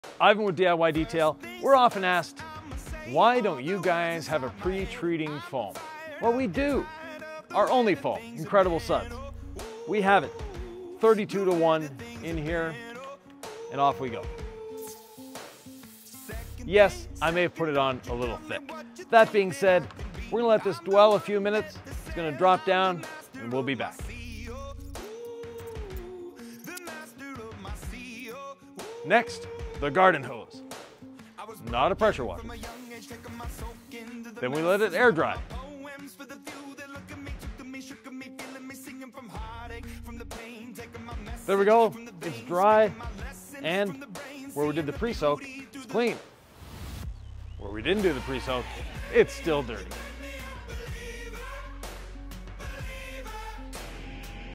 Do you need a dedicated strip wash soap?
Some people say you need a dedicated pre soak/strip wash foam or soap for best cleaning. Is that true?